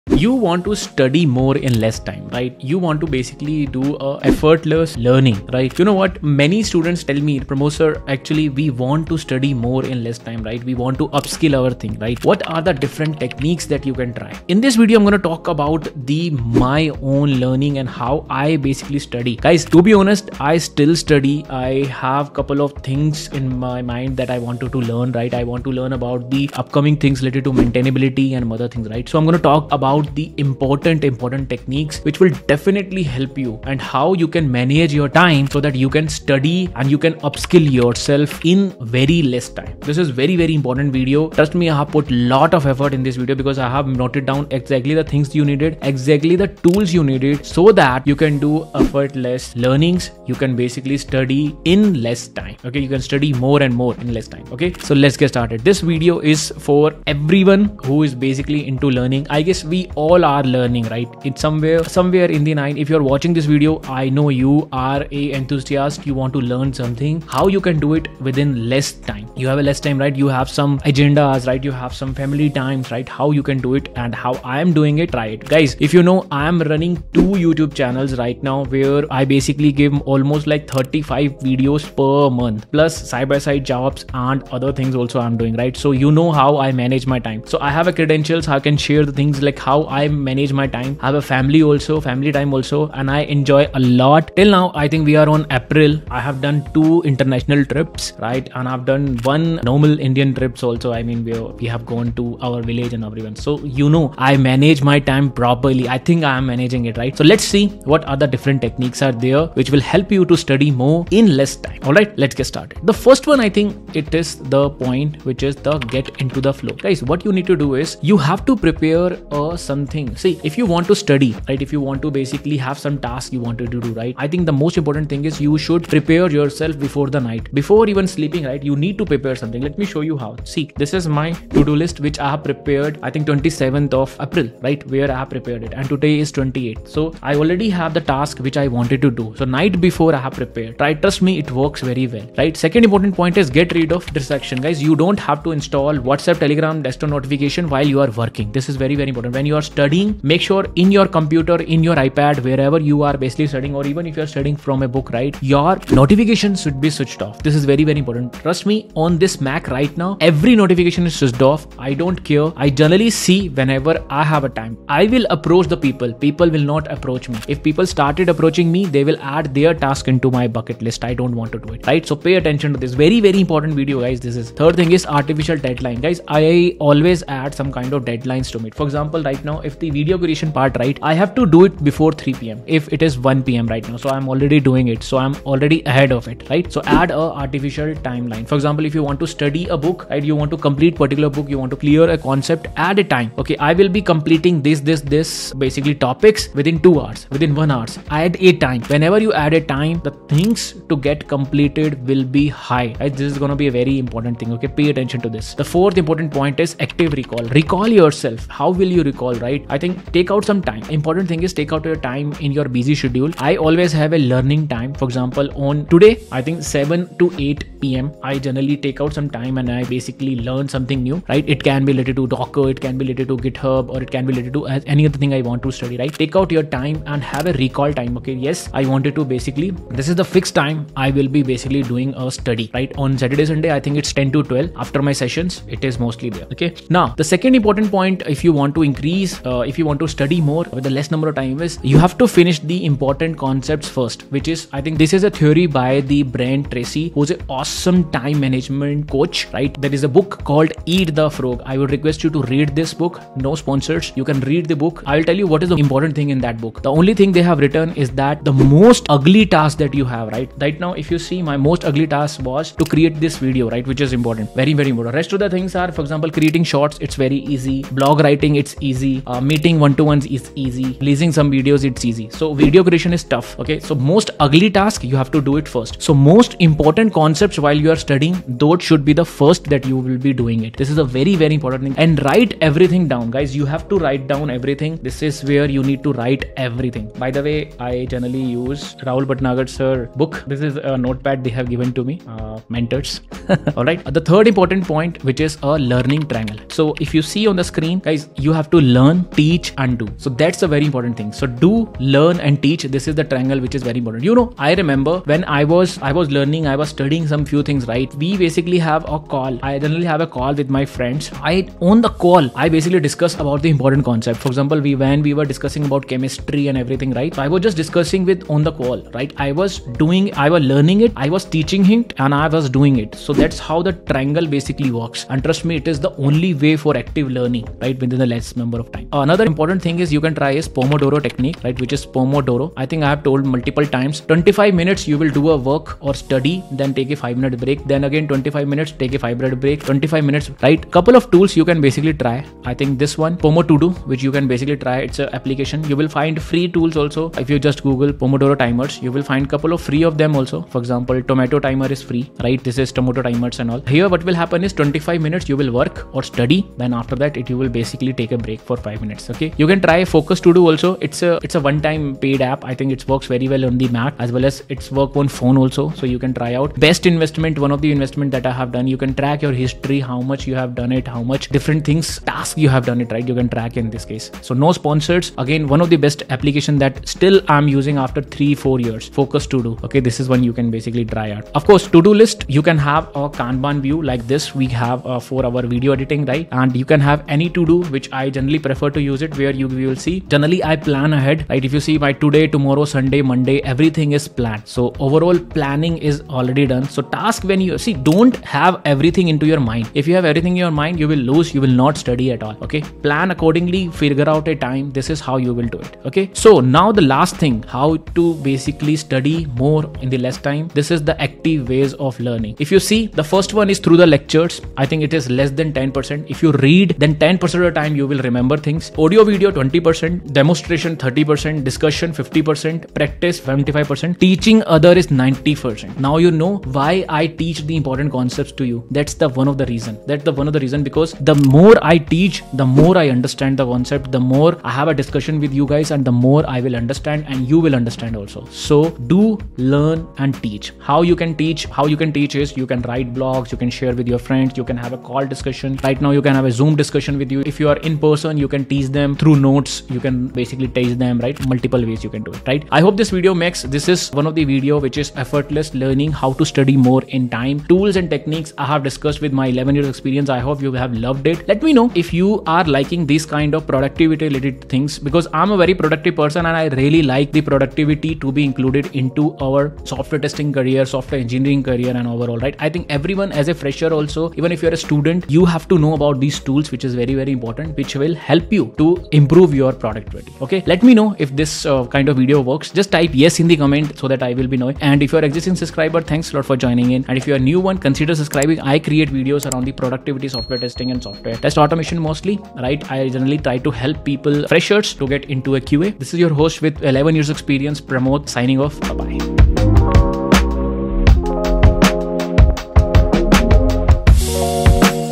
How to Study MORE in LESS Time (Proven Strategy)
Do you struggle with studying and wish you could study more in less time? In this video, TheTestingAcademy shares a study hack that will help you maximize your study time and achieve better results.

This video covers practical tips and strategies to help you study more effectively, including how to manage your time, how to stay motivated, and how to avoid common mistakes that can ruin your study efforts. You'll also learn how to study smart and remember everything you learn.

Whether you're a student preparing for exams or someone looking to improve your productivity, this video is packed with helpful study tips that will help you achieve your goals.

So, if you want to learn how to study more in less time and achieve better results, don't miss this informative video.

🚀 BONUS VIDEO 🚀